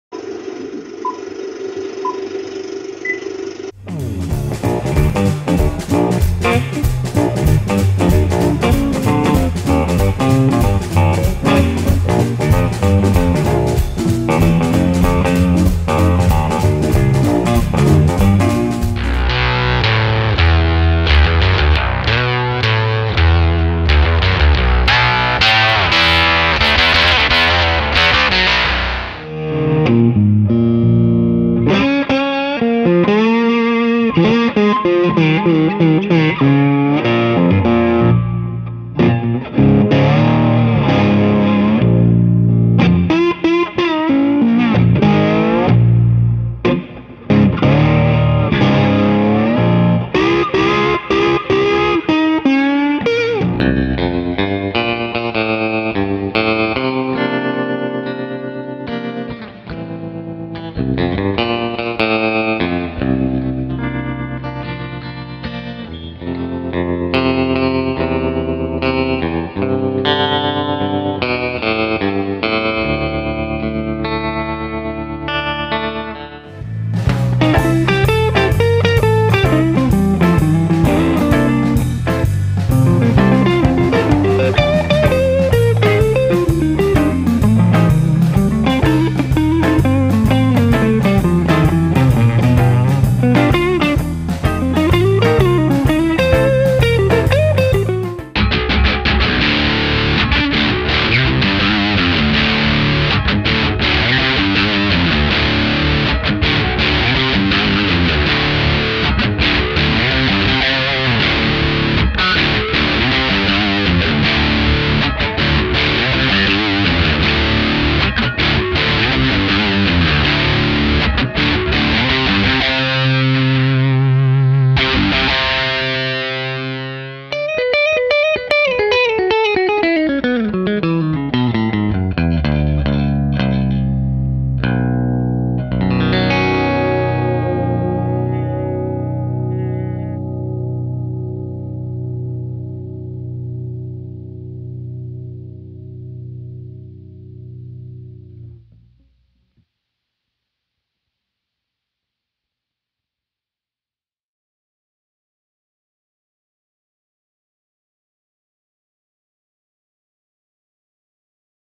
Model One Baritone - Senn by Eastwood
The many different sounds the Senn by Eastwood - Model One Baritone can produce as demonstrated by R.J. Ronquillo and Jeff Senn.